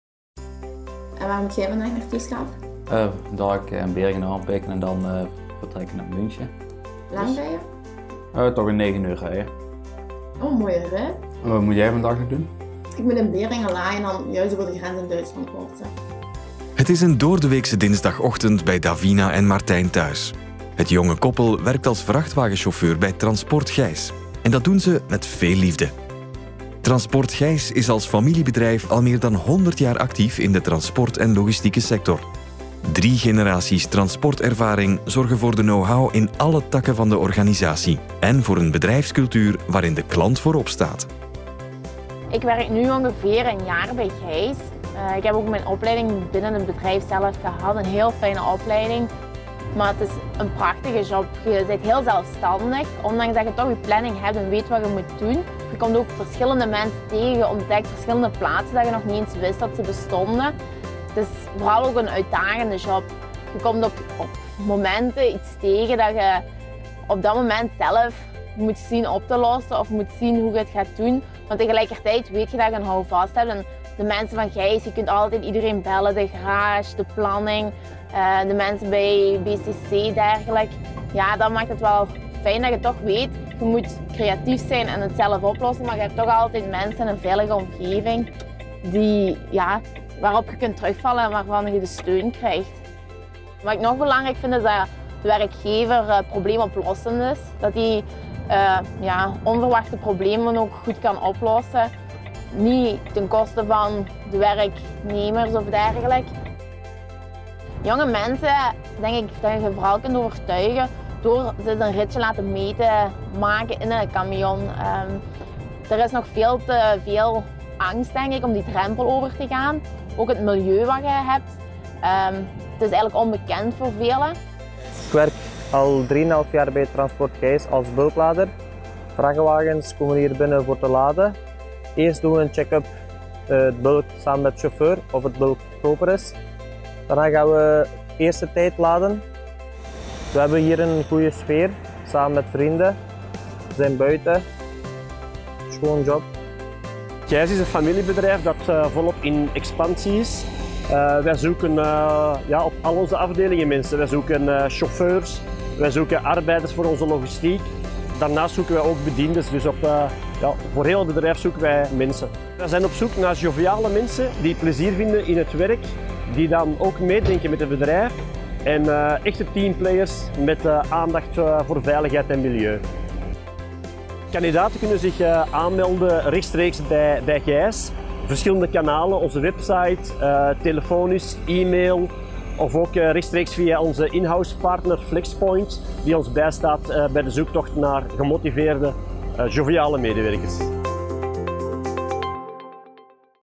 TRANSPORT & VAN.TV 37: Groep Gheys zoekt nieuwe gemotiveerde profielen
HUNTING4JOBS:

Davina Geerdens werkt voor Transport Gheys en is chauffeur in hart en nieren. Samen met haar partner Martijn Mulkens - tevens vrachtwagenchauffeur bij Transport Gheys - vormen ze samen het boegbeeld van de campagne Hunting4Jobs, in de zoektocht naar nieuwe, gemotiveerde collega's.

Transport Gheys is als familiebedrijf al meer dan 100 jaar actief in de transport en logistieke sector. Drie generaties transportervaring zorgen voor de know-how in alle takken van de organisatie. En voor een bedrijfscultuur waarin de klant voorop staat.

In samenwerking met Flexpoint participeerde Gheys Transport & Logistiek aan de nieuwe campagne Hunting4Jobs in de zoektocht naar nieuwe arbeidskrachten. Groep Gheys zoekt profielen in alle departementen van het bedrijf (chauffeurs, arbeiders voor de logistiek en bedienden).